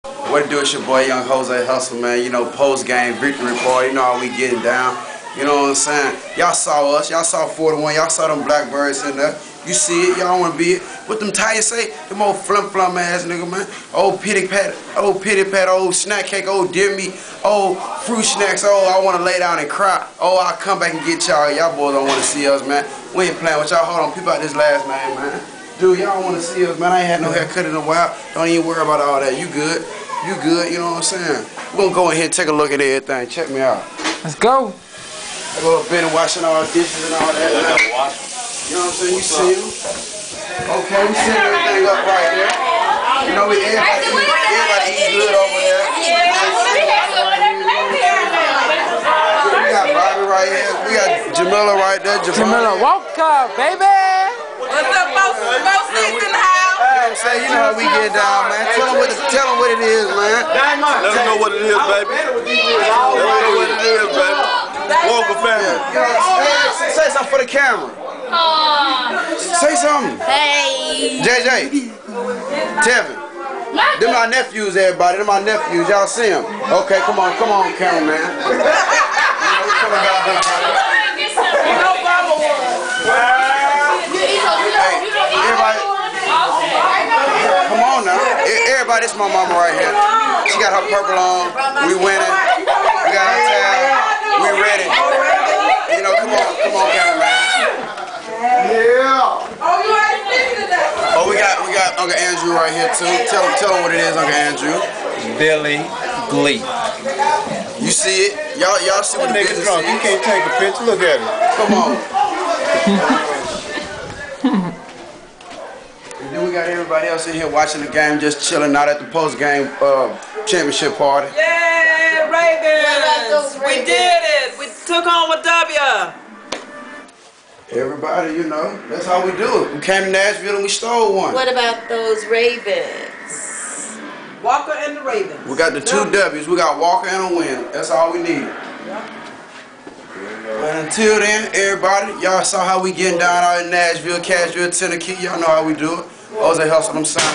Baltimore Ravens 2009 NFL Crazy Playoff Party !!!!
Exclusive Family and Friends Ravens Playoff Party Footage Apperances By: Frank Walker, Ray Lewis, Ed Reed, Troy Smith, Jose Hustle, Derrick Mason And Many More